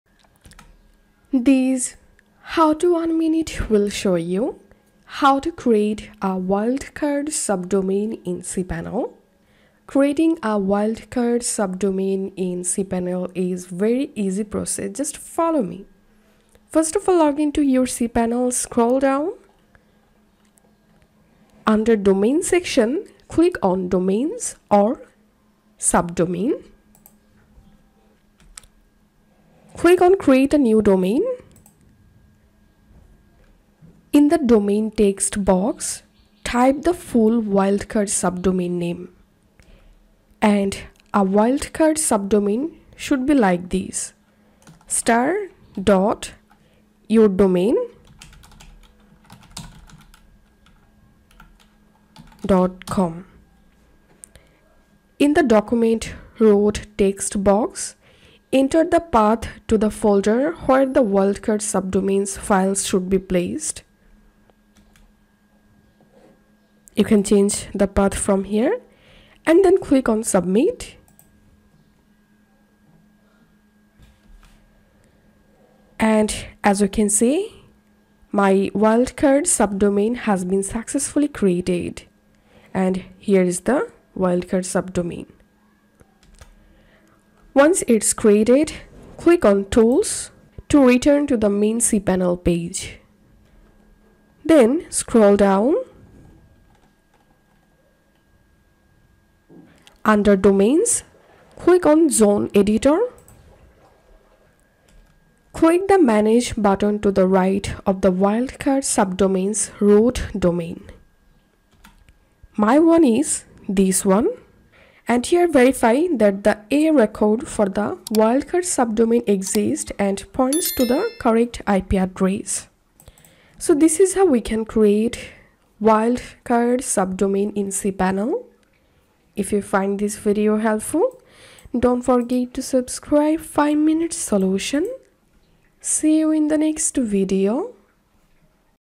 How to create wildcard subdomain in cPanel 2026 | Full Guide
Learn more about How to create a wildcard subdomain in cPanel. Find your answers at the step-by-step video tutorial Knowledge Base.

0:00 Intro
0:20 open domain/sub-domain
0:40 Type full wildcard subdomain
1:02 Setup
2:10 Outro